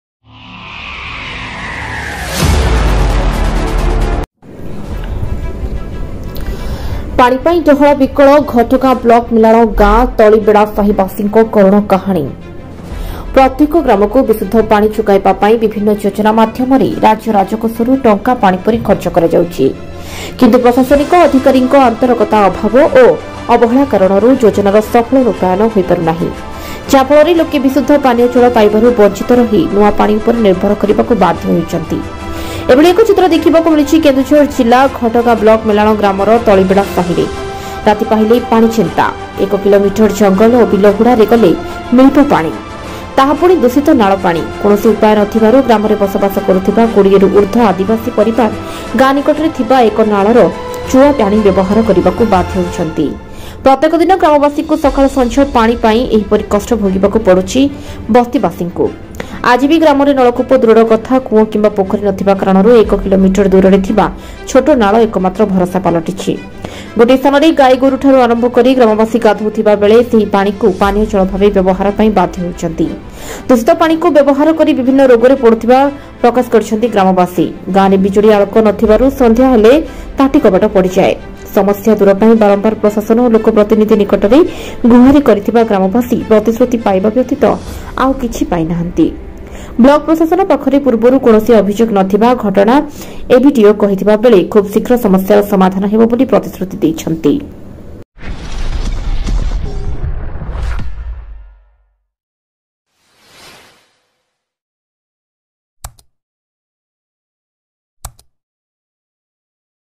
ghatgaon mirror ମେଲାଣ ପାଣି ପାଇଁ ଡହଳ ବିକଳ ହେଉଛନ୍ତି ତଳିବେଡା ବସ୍ଥି ବାସିନ୍ଦା
Video from Journalist Mahendra Patra